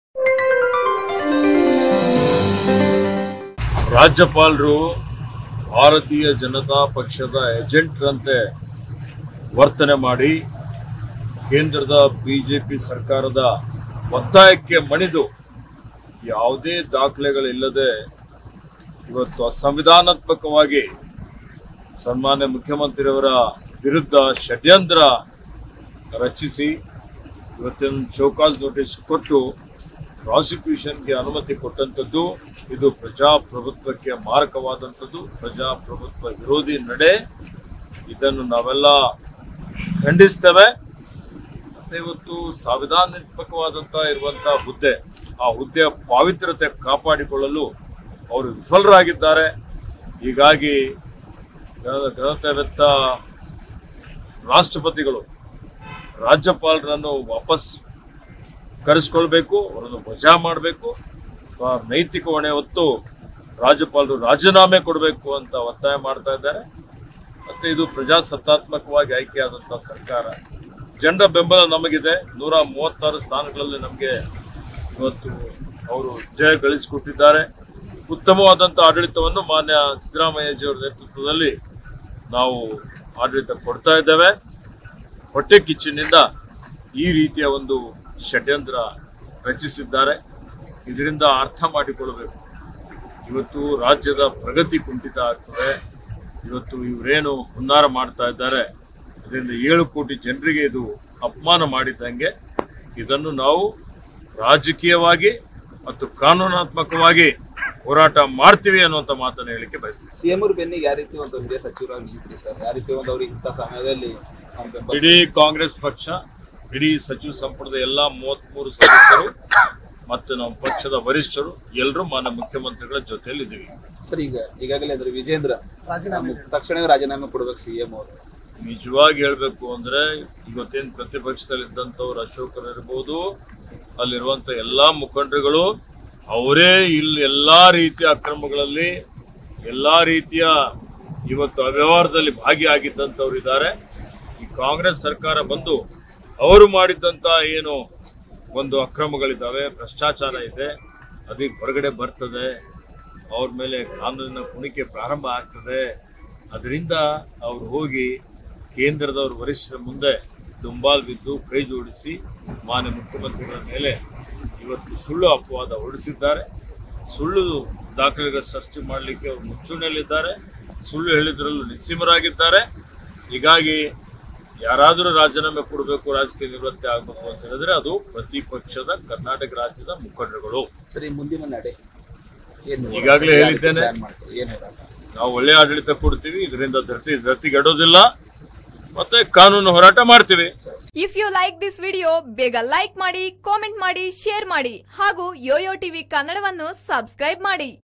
Eshwar Khandre: ಸುಳ್ಳು ದಾಖಲೆ ಸೃಷ್ಟಿ, ಸುಳ್ಳು ಆಪಾದನೆ ಬಿಜೆಪಿಗರು ನಿಸ್ಸೀಮರು | CM Muda Scam | Bidar | YOYO
Minister Eshwar Khandre: ಸುಳ್ಳು ದಾಖಲೆ ಸೃಷ್ಟಿ, ಸುಳ್ಳು ಆಪಾದನೆ ಬಿಜೆಪಿಗರು ನಿಸ್ಸೀಮರು.. | Prosecution against CM Siddaramaiah | CM Muda Scam | Bidar | YOYO TV Kannada

👉 Stay Updated ! 🔔